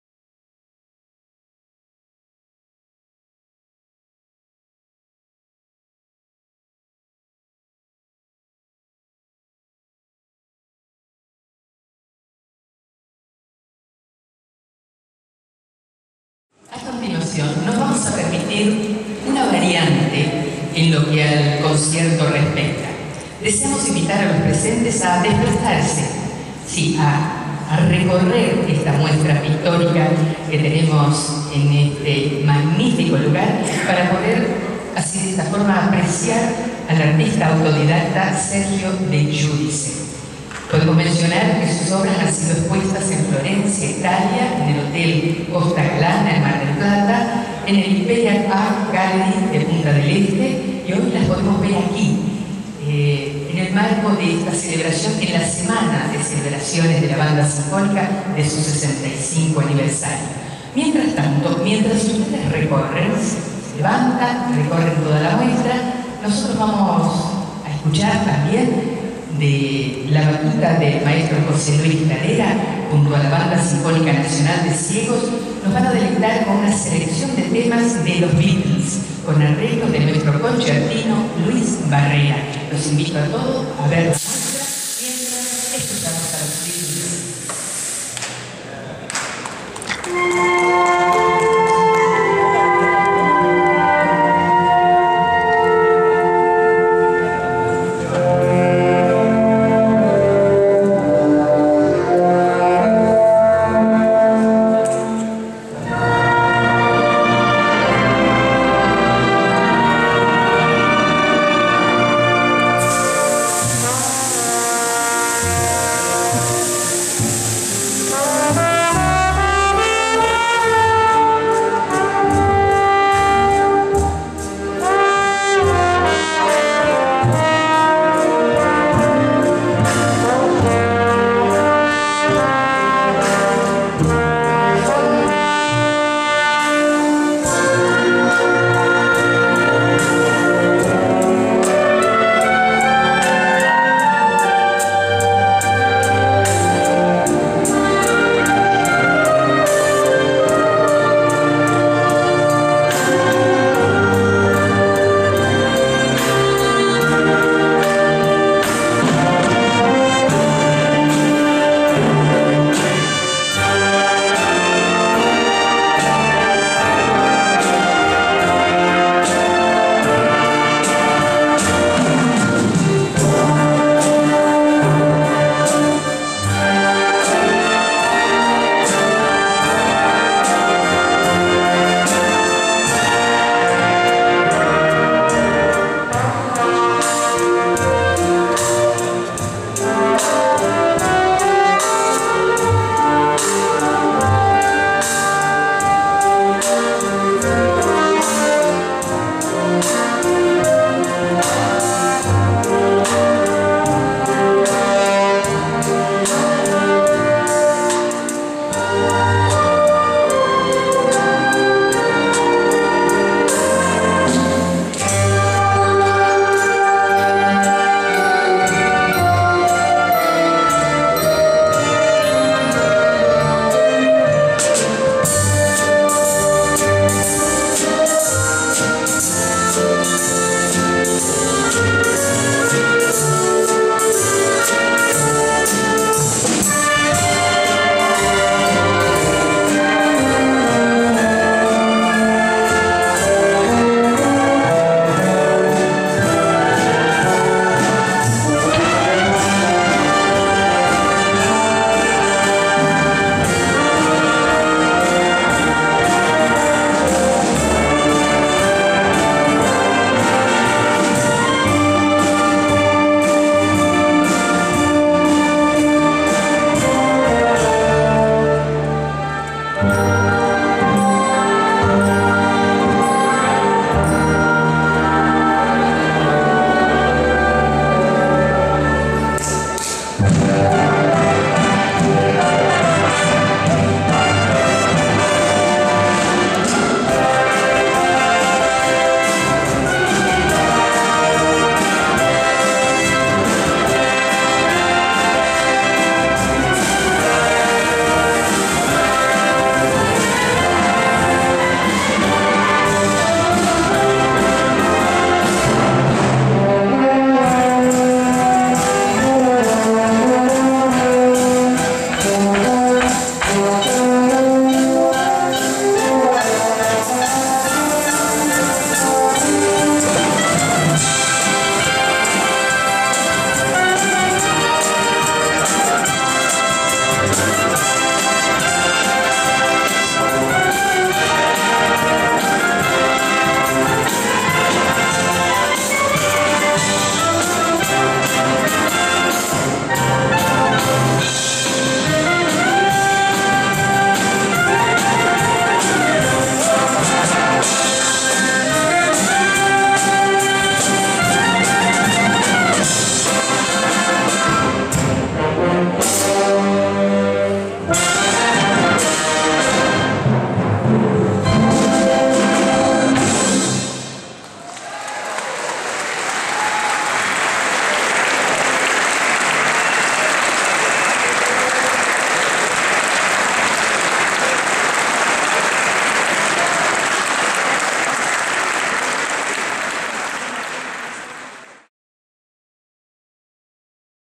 Banda Sinfónica Nacional de Ciegos en la Bolsa de Comercio de Buenos Aires. "The Beatles"
Fragmento del magnifico concierto que La Banda Sinfónica Nacional de Ciegos "PASCUAL GRISOLÍA" ofreciió en la Bolsa de Comercio de la Ciudad de Buenos Aires el dia 19 de Octubre de 2012.
POPURRI DE LOS BEATLES
Director JOSE LUIS CLADERA.